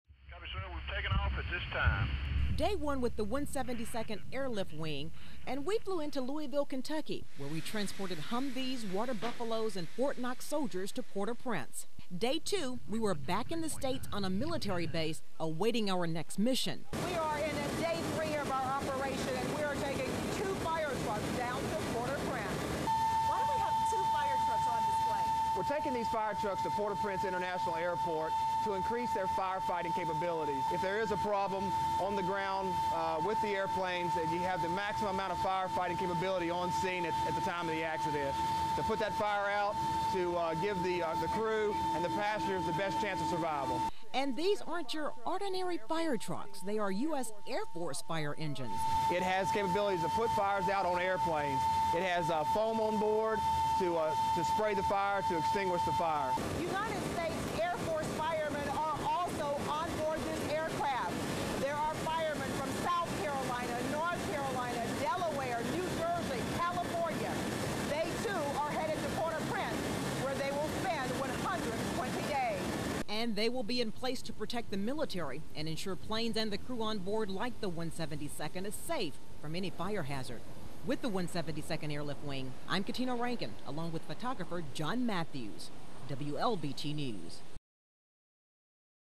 Firetrucks Headed to Haiti
172nd Airlift Squadron takes firetrucks to Haiti